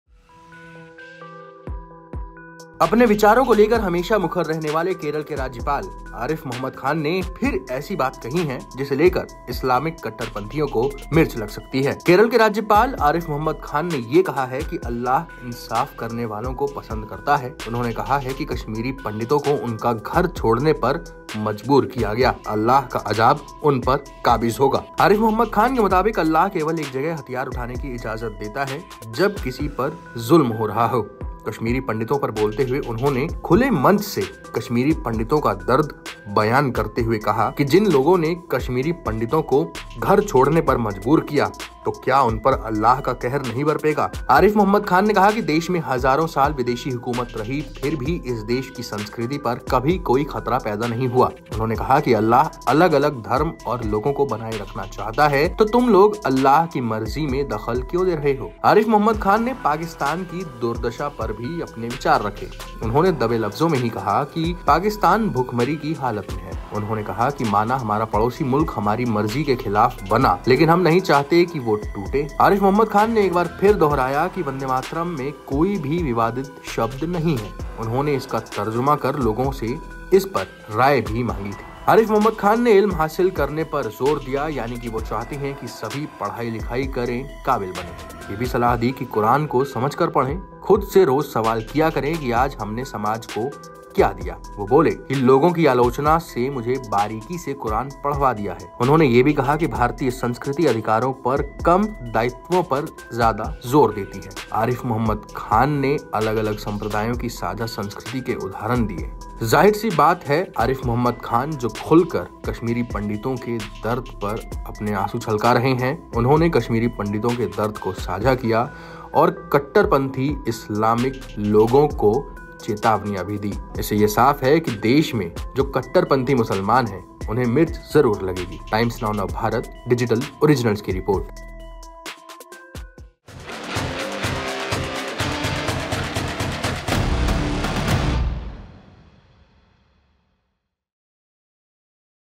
Arif Mohammad Khan ने कहा जिसने Kashmiri Pandit को भगाया उन्हें अल्लाह देखेगा
केरल के राज्यपाल आरिफ़ मोहम्मद ख़ान ने एक बार फिर से ऐसे बयान दिये हैं जिससे कट्टरपंथियों को मिर्च लगने वाली है। इस बार आरिफ़ मोहम्मद ख़ान ने कश्मीरी पंडितों के मुद्दे पर बातें रखीं।